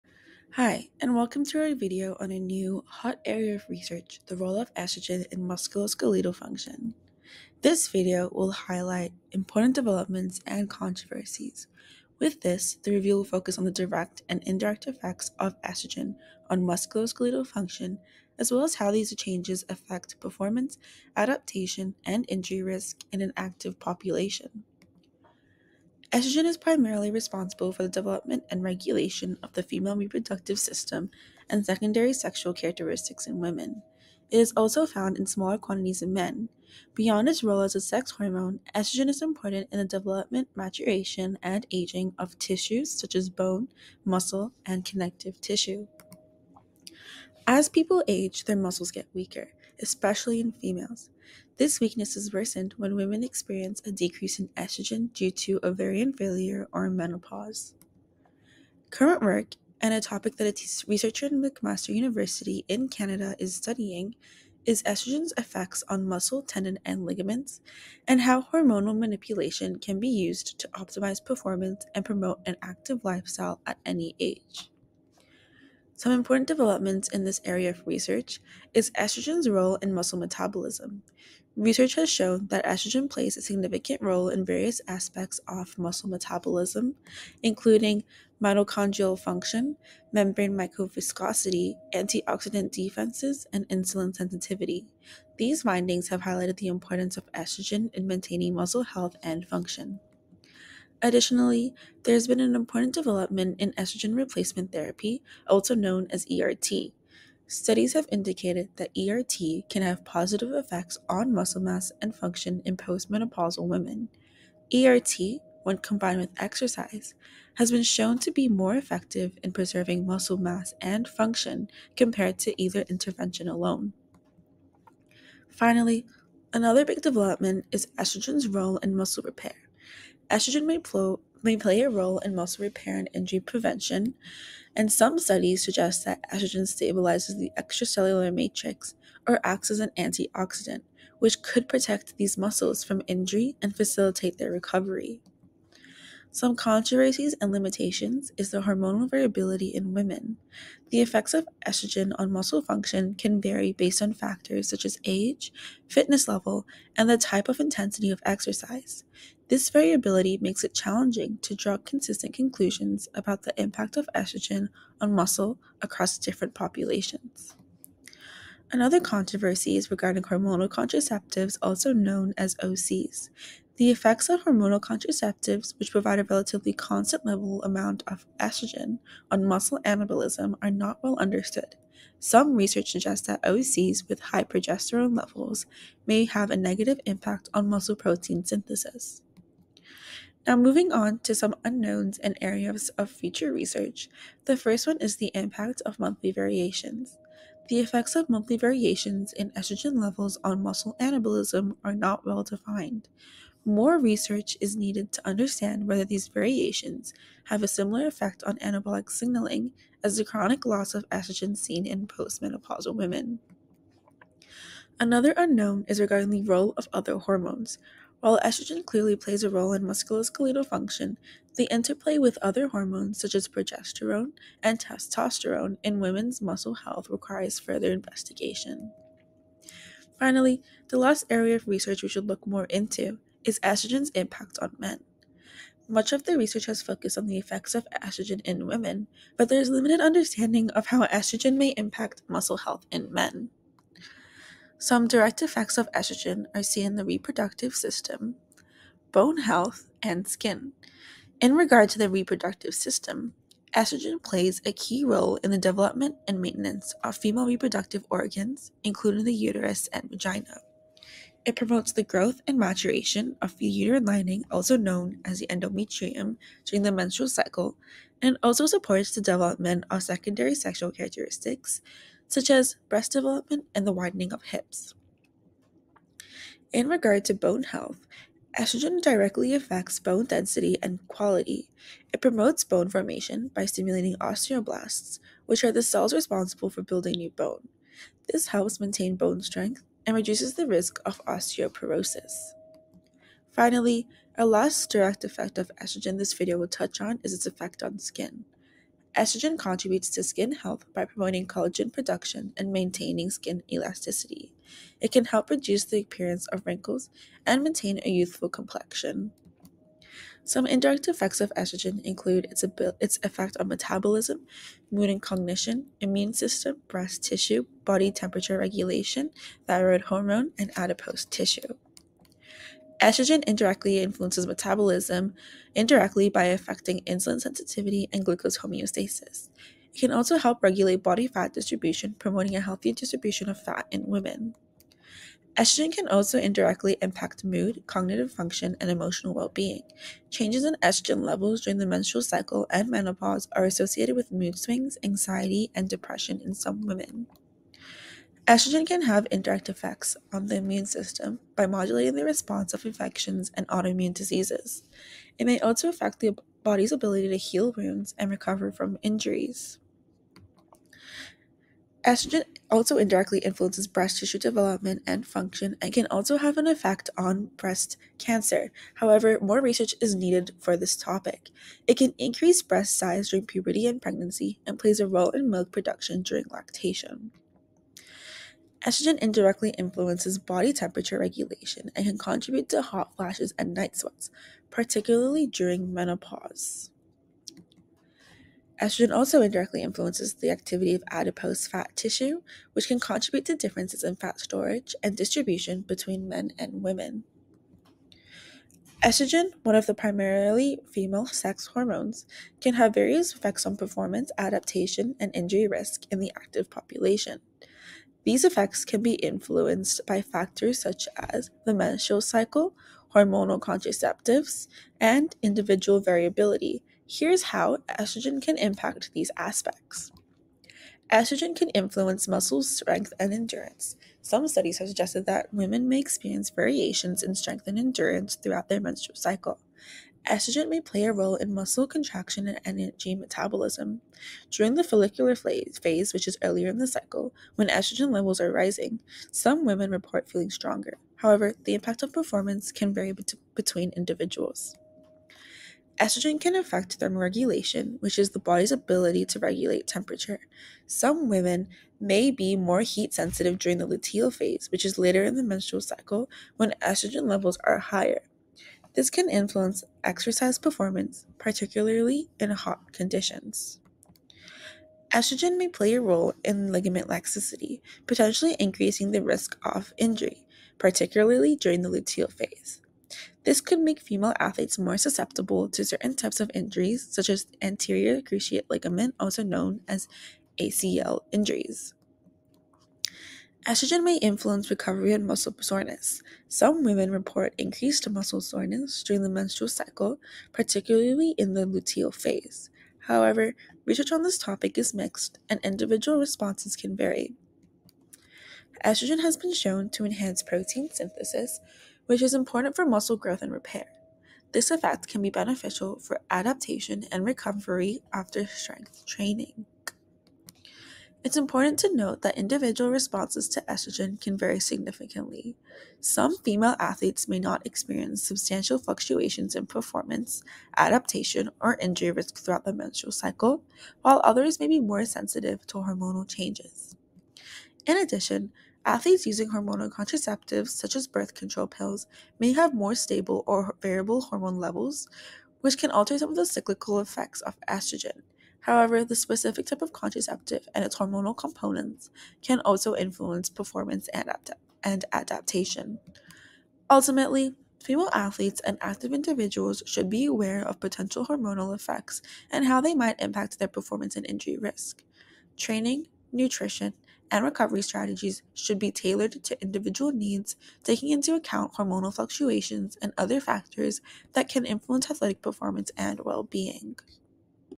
Estrogen Effect on Muscle
Welcome to an insightful video exploring the multifaceted role of estrogen in musculoskeletal function. Delving into recent developments and controversies, we investigate both direct and indirect effects of estrogen on tissues such as bone, muscle, and connective tissues. Notably, research from McMaster University highlights estrogen's impact on muscle, tendons, and ligaments, emphasizing the potential for hormonal manipulation to optimize performance and active lifestyles across ages. The review covers Estrogen's Role in Muscle Metabolism, Estrogen Replacement Therapy, and its role in muscle repair. Despite progress, challenges persist, including hormonal variability in women and uncertainties surrounding hormonal contraceptives. The video concludes by addressing unknowns, such as the impact of monthly variations and the role of other hormones, urging further exploration, especially in understanding estrogen's impact on men. The presentation underscores the need for tailored strategies in training and nutrition, considering individual responses and hormonal fluctuations, to optimize athletic performance and well-being in the active population.

This video was made my McMaster University students Asha Mundi, Esha Bhutkar, Iman Malik, and Palvir Nagra in collaboration with the Demystifying Medicine McMaster Program.

We would also like to thank Dr.Takuma Isshiki, Brooke D’Mello, and Safaa Naiel for providing feedback and reviewing the contents of this video.

References:
Chidi-Ogbolu, N., & Baar, K. (2019). Effect of Estrogen on Musculoskeletal Performance and Injury Risk. Frontiers in physiology, 9, 1834.

Adachi, N., Nawata, K., Maeta, M., & Kurozawa, Y. (2008). Relationship of the menstrual cycle phase to anterior cruciate ligament injuries in teenaged female athletes. Archives of orthopaedic and trauma surgery, 128(5), 473–478.

Delgado BJ, Lopez-Ojeda W. Estrogen. [Updated 2023 Jun 26]. In: StatPearls [Internet]. Treasure Island (FL): StatPearls Publishing; 2023 Jan-.

Lobo RA, Pickar JH, Stevenson JC, Mack WJ, Hodis HN. Back to the future: Hormone replacement therapy as part of a prevention strategy for women at the onset of menopause. Atherosclerosis. 2016 Nov;254:282-290.

Lobo RA. Hormone-replacement therapy: current thinking. Nat Rev Endocrinol. 2017 Apr;13(4):220-231.

Bednarska S, Siejka A. The pathogenesis and treatment of polycystic ovary syndrome: What's new? Adv Clin Exp Med. 2017 Mar-Apr;26(2):359-367.

Shumaker SA, Legault C, Rapp SR, Thal L, Wallace RB, Ockene JK, Hendrix SL, Jones BN, Assaf AR, Jackson RD, Kotchen JM, Wassertheil-Smoller S, Wactawski-Wende J., WHIMS Investigators. Estrogen plus progestin and the incidence of dementia and mild cognitive impairment in postmenopausal women: the Women's Health Initiative Memory Study: a randomized controlled trial. JAMA. 2003 May 28;289(20):2651-62.